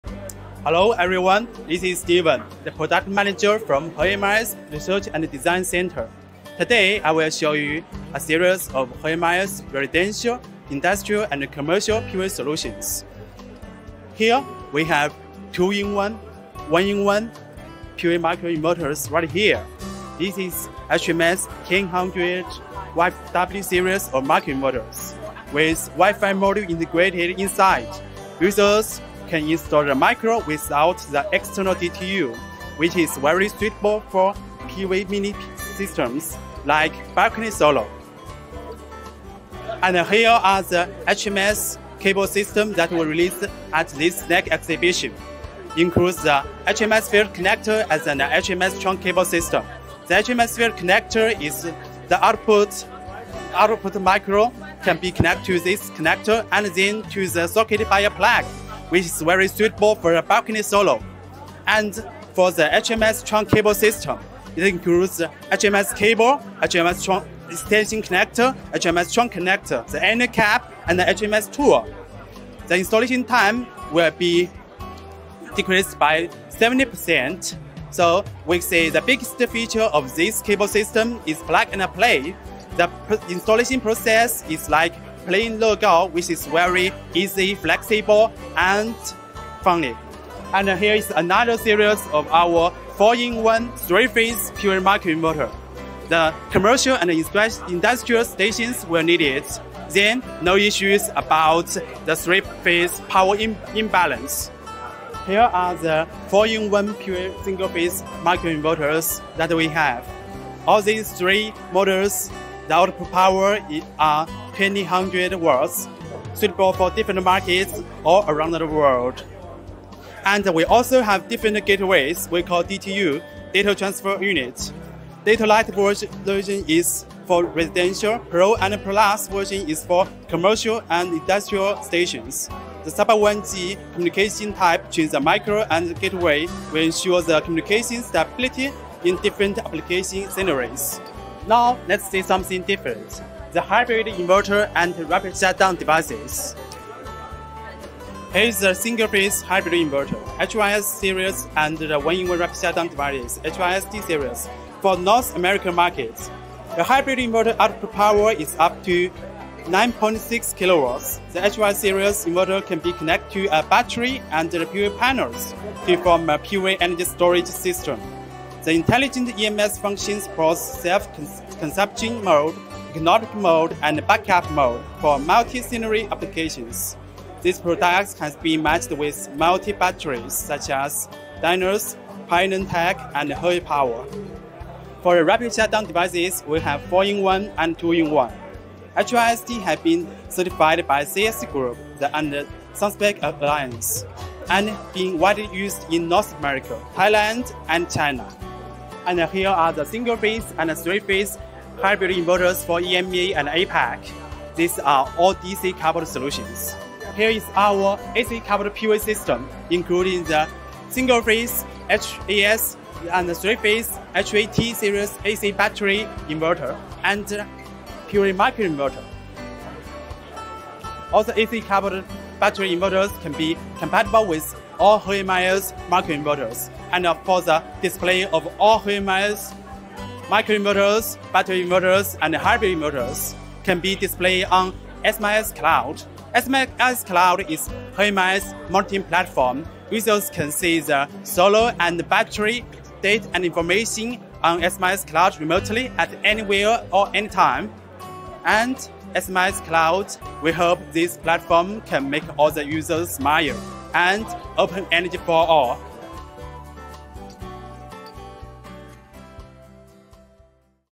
Exploring Hoymiles Solar Solutions
Discover the exciting range of Hoymiles' products showcased at 2023SNEC! Steven, Hoymiles' Product Director, introduces our lineup at our booth.

We're proud to present our solar solutions, including the newly released FLEXseries microinverter installation solution, HMS and HMT series of microinverters and hybridinverters. Watch the video to take a closer look at these remarkable products.

PS. A big THANK YOU to PV Tech for their exceptional filming and editing work in bringing this video to life!